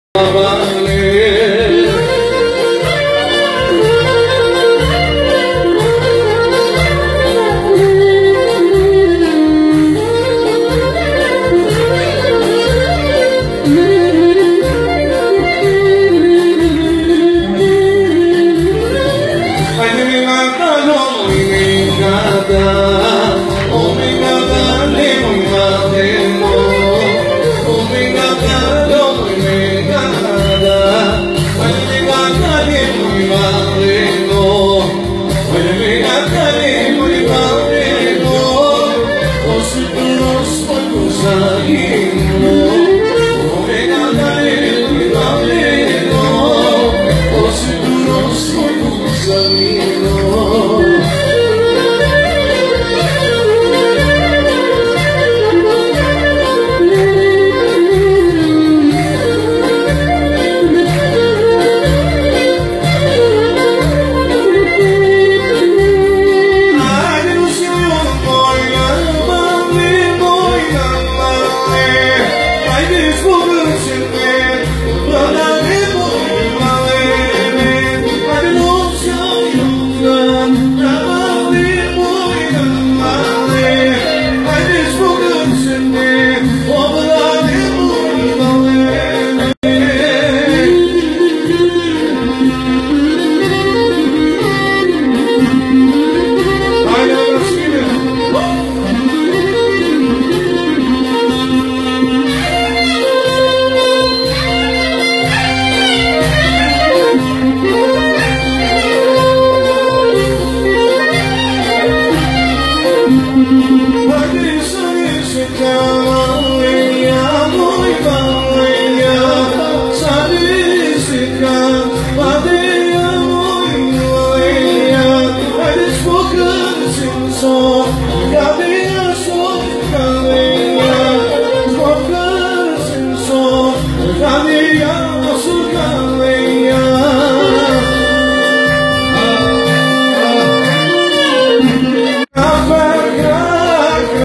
Dasëm në Fier Liveeee "2018"Mikel Rapaj $ Polo $ Ervis Rapaj $ Stavri Lula.
Për Dasma dhe Gëzime këndon:
Mikel Rapaj&Polo
Klarinet:Ervis Rapaj
Organo:Stavri Lula
Ju faleminderit.!